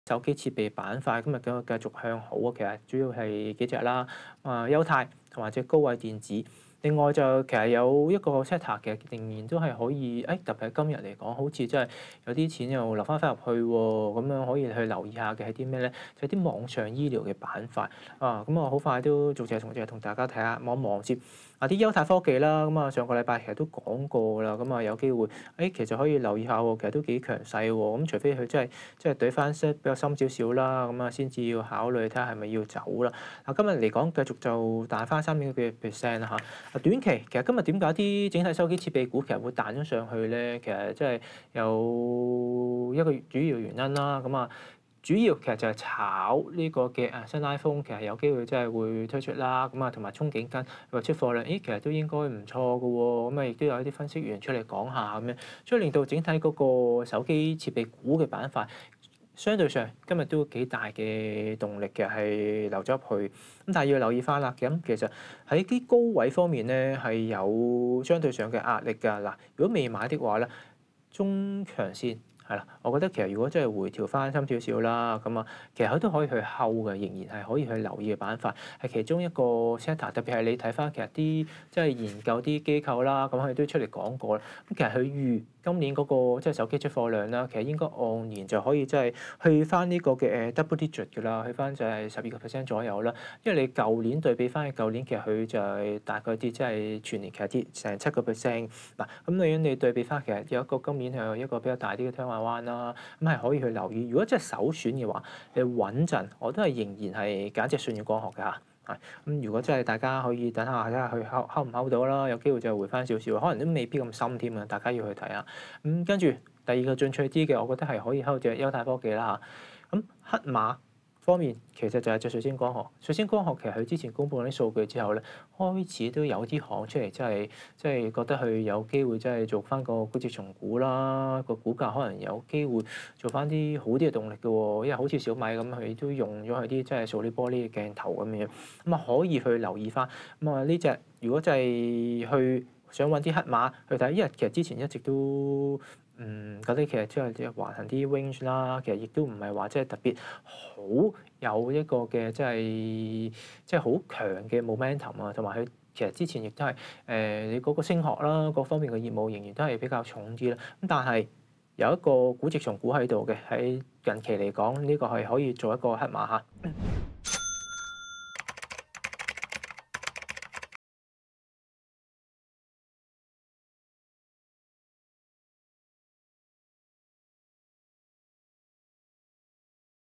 【ET講股通】（精華）手機設備股彈升 想買入應如何部署？
有報道指iPhone13有望支持屏幕指紋解鎖，預計8月底、9月初投入量產，手機設備股今日彈升，想買入可以如何部署？梁耀康為你分析。